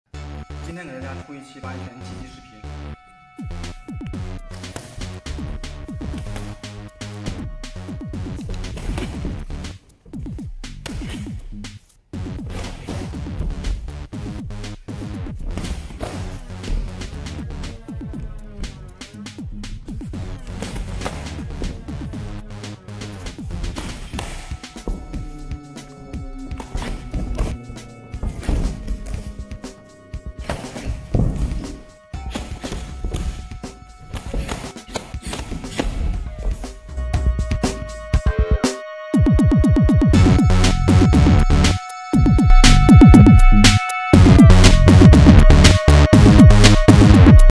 Mengcun Baji Quan : fight applications
Some fight applications demonstrated by Zhang Nan, one of the trainers of the Mengcun Baji Quan school ...